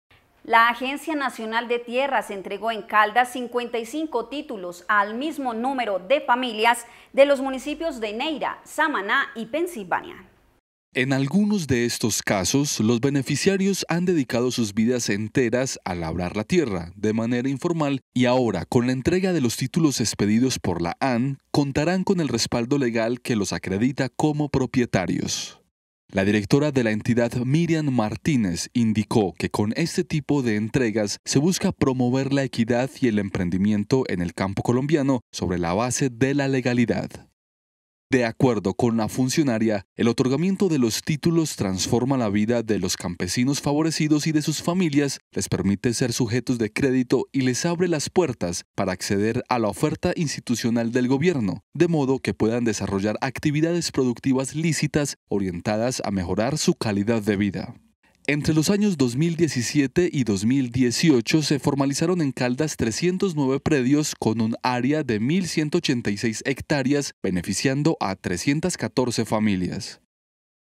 55 FAMILIAS DE CALDAS YA SON PROPIETARIAS DE SUS TIERRAS
La Agencia Nacional de Tierras entregó en Caldas 55 títulos de propiedad a igual número de familias de los municipios de Neira, Samaná y Pensilvania.